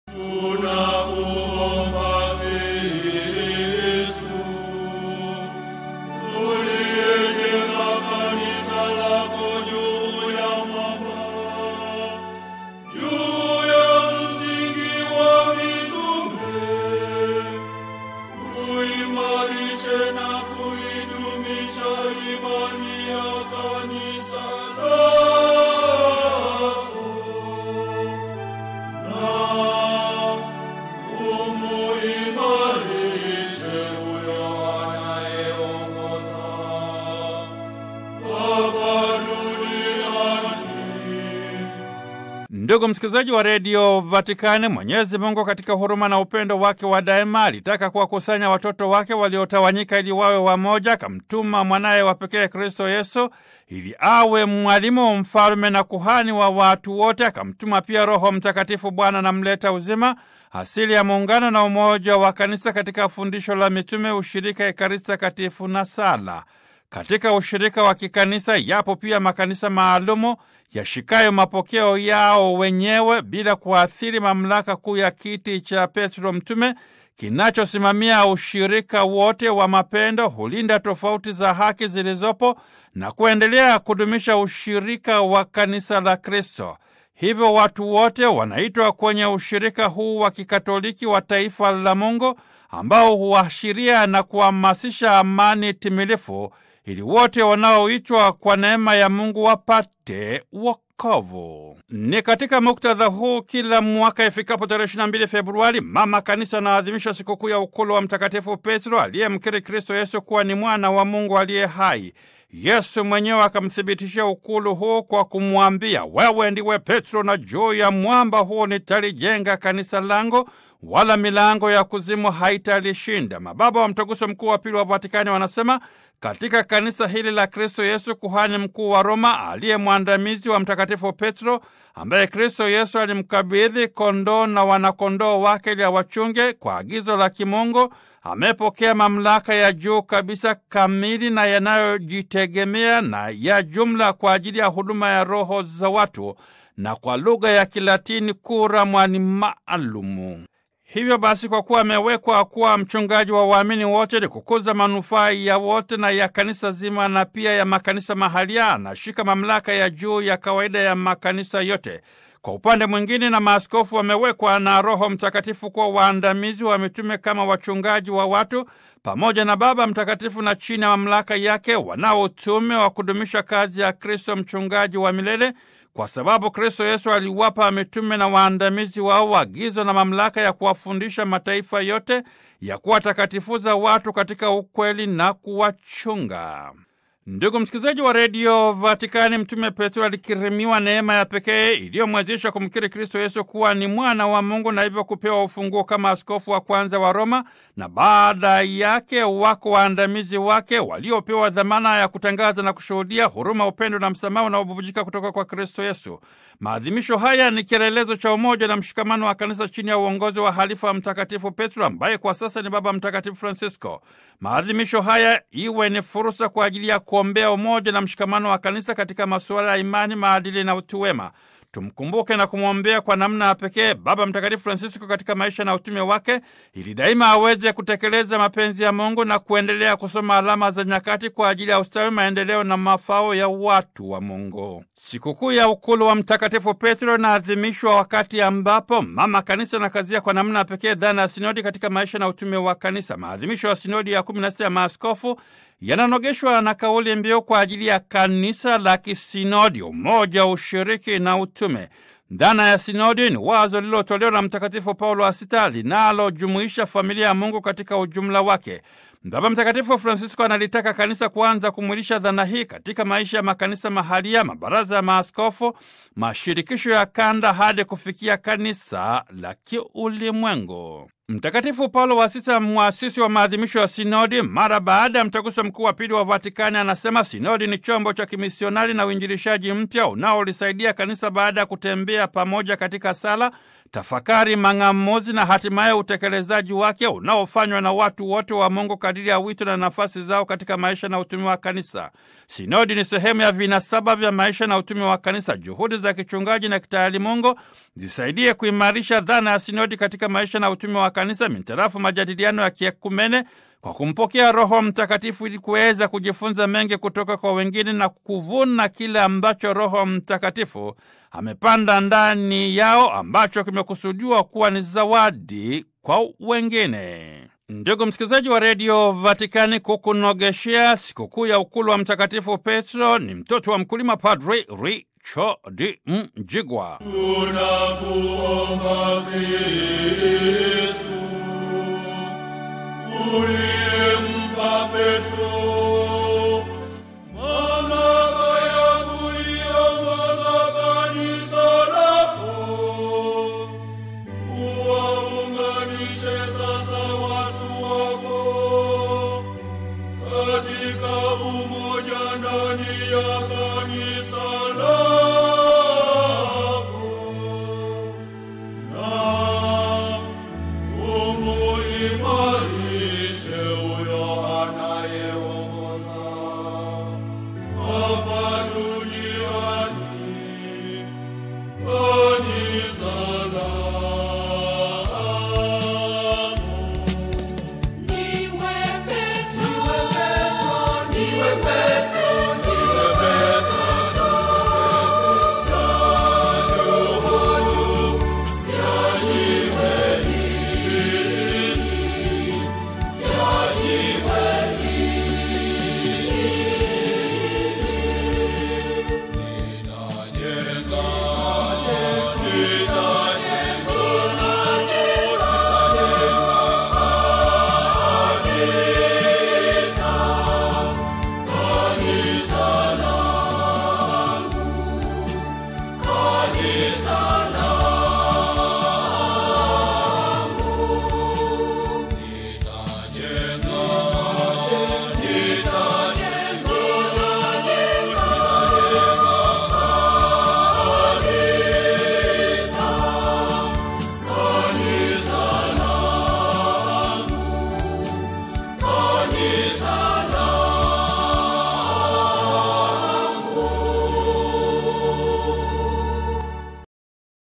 Sikukuu ya Ukulu wa Mtume Petro: Kanisa la Kisinodi: Umoja, Ushiriki na Utume.
Sikukuu ya Ukulu wa Mtakatifu Petro inaadhimishwa wakati ambapo Mama Kanisa anakazia kwa namna ya pekee dhana ya Sinodi katika maisha na utume wa Kanisa. Maadhimisho ya Sinodi ya XVI ya Maaskofu yananogeshwa na kauli mbiu “Kwa Ajili ya Kanisa la Kisinodi: Umoja, Ushiriki na Utume." Papa analitaka Kanisa kuanza kumwilisha dhana hii katika maisha ya Makanisa mahalia.
Na Padre Richard A. Mjigwa, C.PP.S. – Vatican.

Mwenyezi Mungu katika huruma na upendo wake wa daima alitaka kuwakusanya watoto wake waliotawanyika ili wawe wamoja. Akamtuma Mwanaye wa Pekee Kristo Yesu, ili awe: Mwalimu, Mfalme na Kuhani wa watu wote. Akamtuma pia Roho Mtakatifu, Bwana na Mleta uzima, asili ya muungano na umoja wa Kanisa katika: fundisho la Mitume, Ushirika, Ekaristi Takatifu na Sala. Katika ushirika wa Kikanisa yapo pia Makanisa maalum (Ecclesiae particulares) yashikayo mapokeo yao wenyewe, bila kuathiri mamlaka kuu ya Kiti cha Petro Mtume kinachosimamia ushirika wote wa mapendo, hulinda tofauti za haki zilizopo na kuendelea kudumisha ushirika wa Kanisa la Kristo. Hivyo, watu wote wanaitwa kwenye ushirika huu wa Kikatoliki wa Taifa la Mungu, ambao huashiria na kuhamasisha amani timilifu, ili wote wanaoitwa kwa neema ya Mungu wapate wokovu. Rej. LG 13. Ni katika muktadha huu, Kila mwaka ifikapo tarehe 22 Februari, Mama Kanisa anaadhimisha Sikukuu ya Ukulu wa Mtakatifu Petro, aliyemkiri Kristo Yesu kuwa ni Mwana wa Mungu aliye hai. Yesu mwenyewe akamtibitishia Ukulu huu kwa kumwambia “Wewe ndiwe Petro na juu ya Mwamba huu nitalijenga Kanisa langu, wala milango ya kuzimu haitalishinda”. Mt. 16:18.

Mababa wa Mtaguso Mkuu wa Pili wa Vatican wanasema, katika Kanisa hili la Kristo Yesu, Kuhani Mkuu wa Roma, aliye mwandamizi wa Mtakatifu Petro, ambaye Kristo Yesu alimkabidhi kondoo na wanakondoo wake ili awachunge, kwa agizo la kimungu amepokea mamlaka ya juu kabisa, kamili na yanayojitegemea na ya jumla kwa ajili ya huduma ya roho za watu “Curam animalum”. Hivyo basi, kwa kuwa amewekwa kuwa mchungaji wa waamini wote, ili kukuza manufaa ya wote na ya Kanisa zima na pia ya Makanisa mahalia, anashika mamlaka ya juu ya kawaida juu ya Makanisa yote. Kwa upande mwingine na Maaskofu wamewekwa na Roho Mtakatifu kuwa waandamizi wa Mitume kama wachungaji wa watu, na pamoja na Baba Mtakatifu na chini ya Mamlaka yake, wanao utume wa kudumisha kazi ya Kristo Mchungaji wa milele, kwa sababu Kristo Yesu aliwapa Mitume na waandamizi wao agizo na mamlaka ya kuwafundisha mataifa yote, ya kuwatakatifuza watu katika ukweli na kuwachunga. Rej. Christus Dominum, n. 2-3.

𝕄𝕒𝕨𝕒𝕤𝕚𝕝𝕚𝕒𝕟𝕠:
JUGO MEDIA,